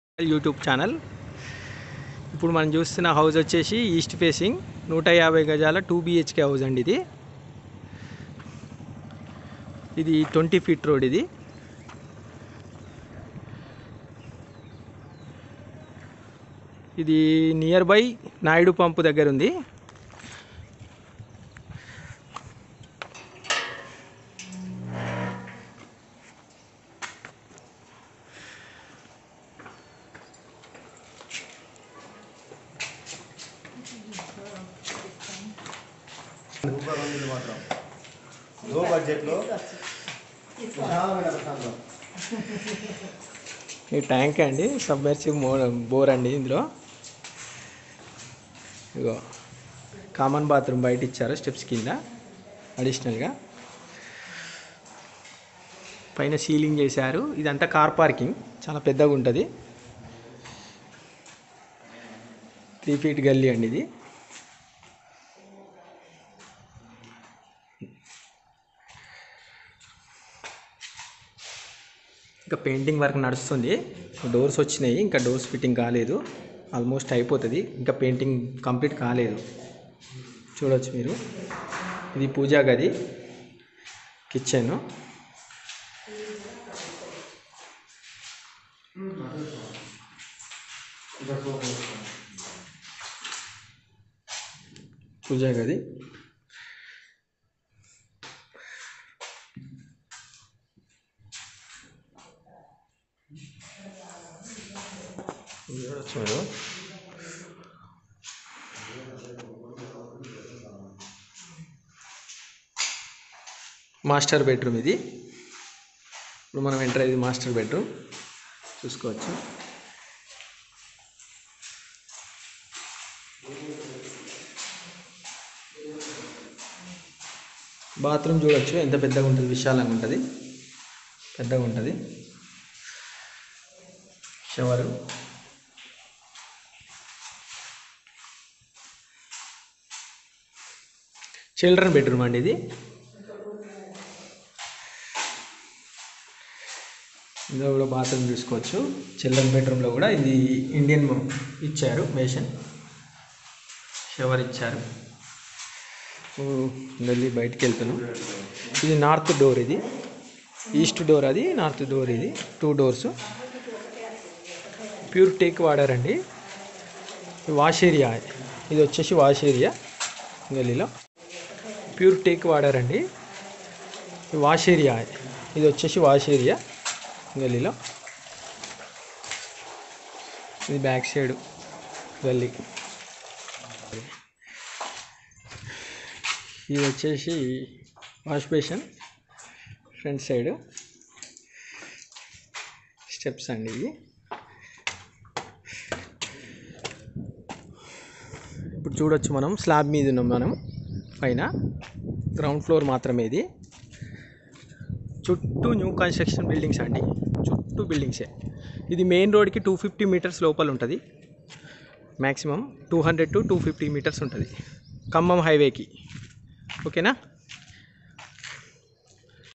Beautiful Independent house sale in Warangal
Naidu pump near
150 sqr yards New house for sale,
East facing,
2bhk,
Boar undhi,
20ft road ,
67 lakhs out rate,

👉👉👉🔥Agent commission2% mandatory ga buyers pay cheyali,